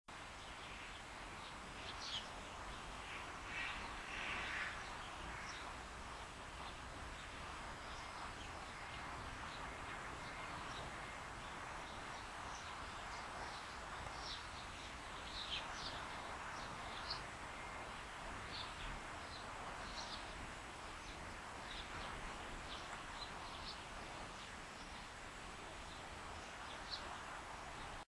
セキレイ
テケテケ歩くセキレイ